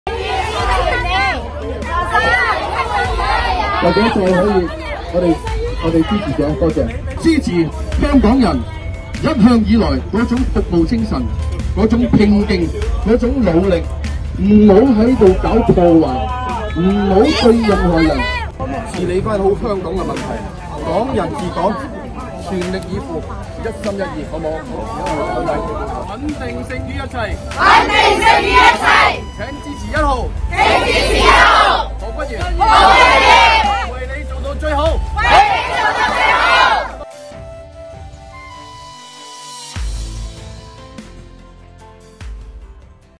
HK legislator showed up at a campaigning event
HongKong legislator Junius Ho Kwan-yiu showed up at a campaigning event on Sunday after he was recently discharged from a hospital. He was attacked by a knife-wielding man on Wednesday morning.